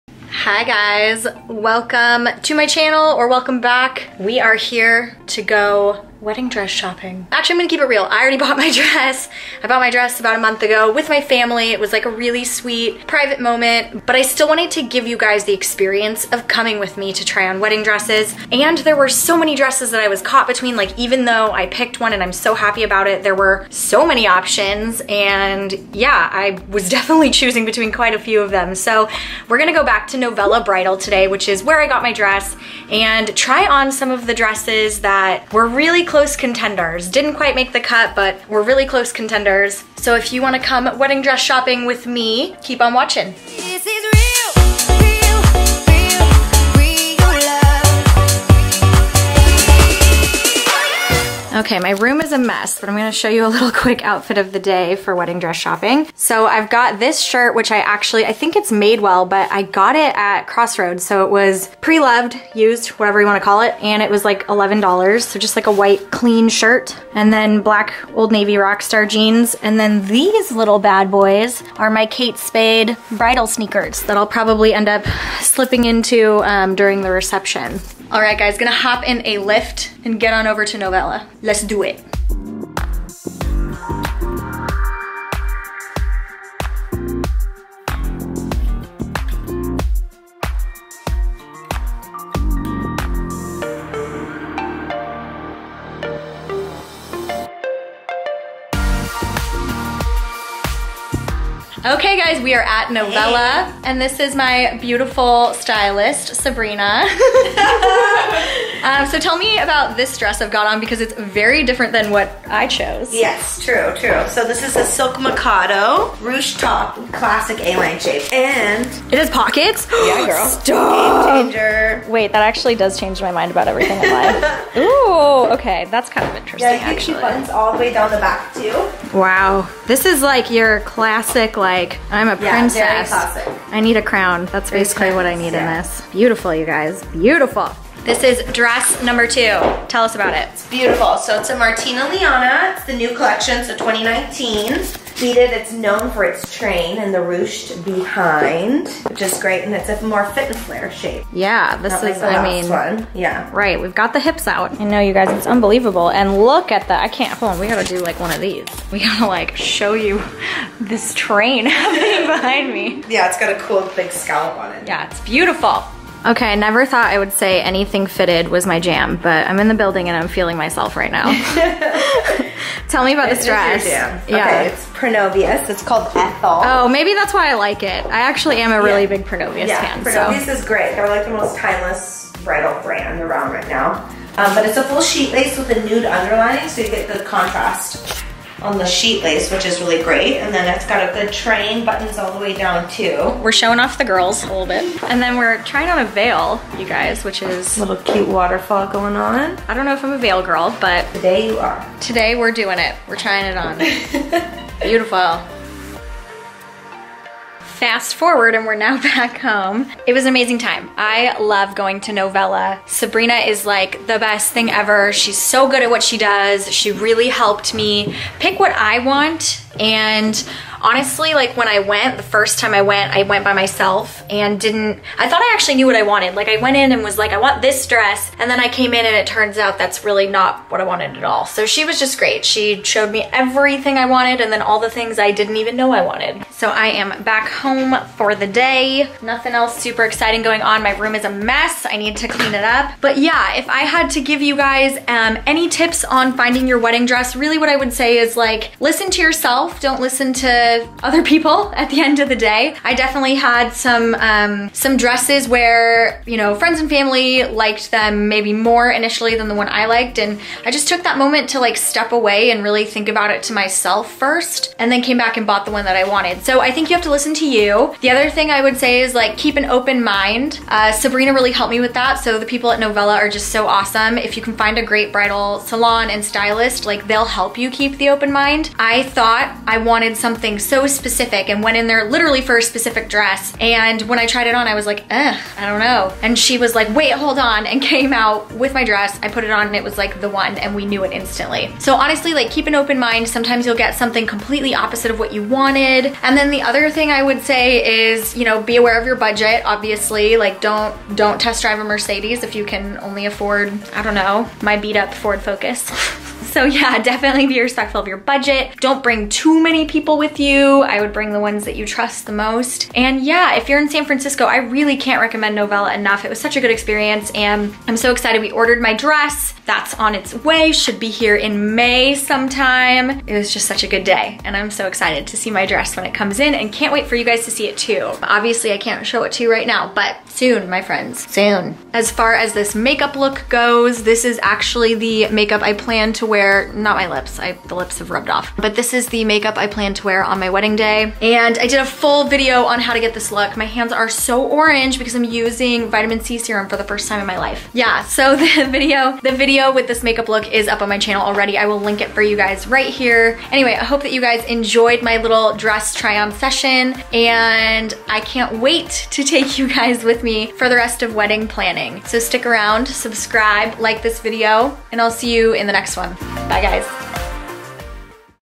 TRYING ON WEDDING DRESSES | Blondes & Bagels
Let's go WEDDING DRESS SHOPPING! Come with me as I vlogged my experience trying on wedding dresses at Novella Bridal in San Francisco! Which one is your favorite?

LET'S BE REAL FRIENDS